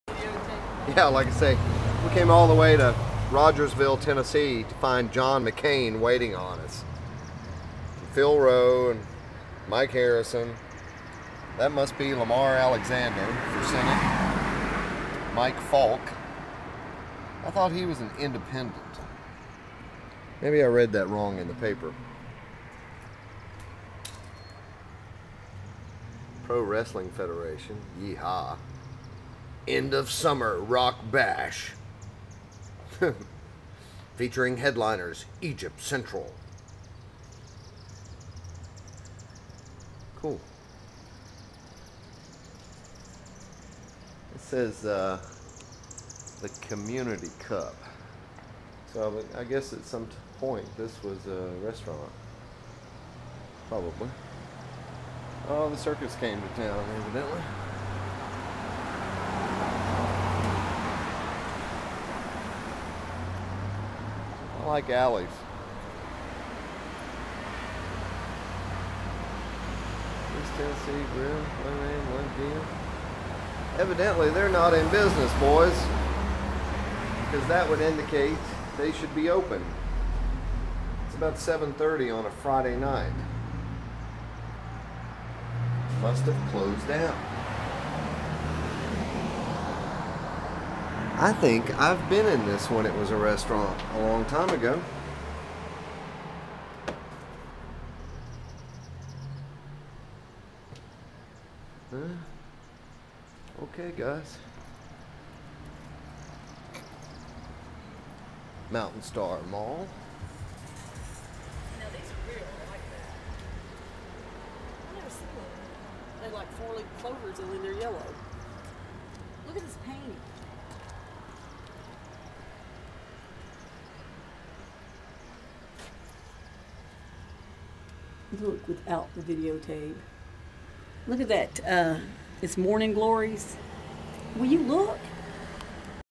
Trip to Rogersville 9-19-08 (20)
Trip to Rogersville 9-19-08. We came all the way to Rogersville and found John McCain waiting on us. Damn! And there are some empty store fronts along the main street -- I'm not sure why since Rogersville is such a quaint little town. I love the sidewalk here.